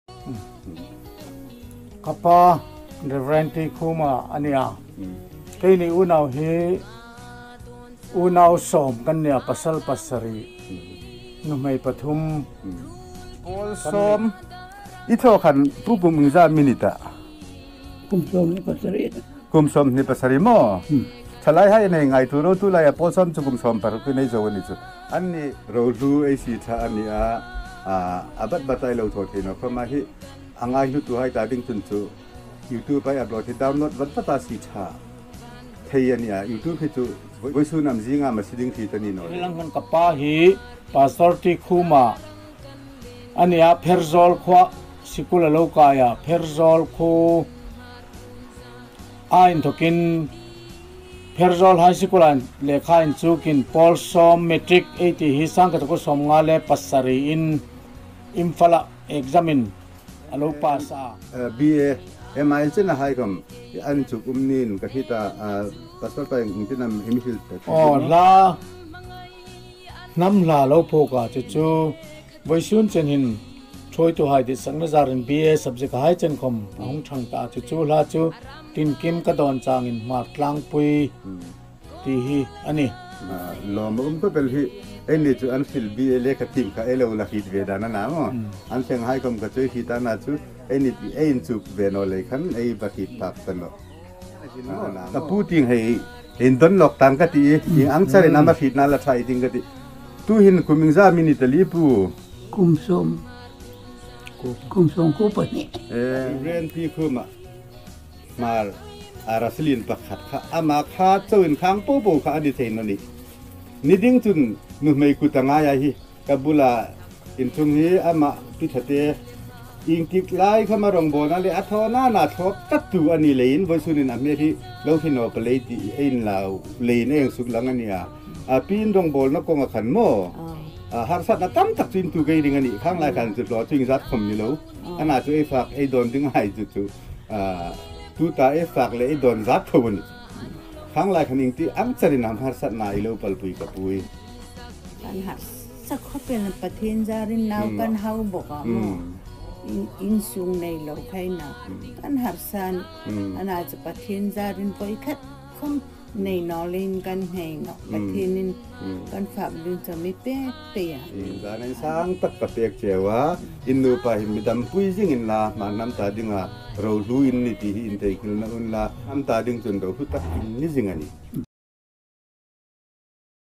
An Interview with Rev T Khuma | Song Writer and Composer | Trailer
An Interview with Rev T Khuma | Song Writer and Composer

Hi Video hi Hmar Arasi lien pakhat, Hmar hnam ta ding le Pathien ta dinga thawhlawk em em Rev T Khuma le Hohlimna a nih a, Full interview ei en thei vat beisei a nih.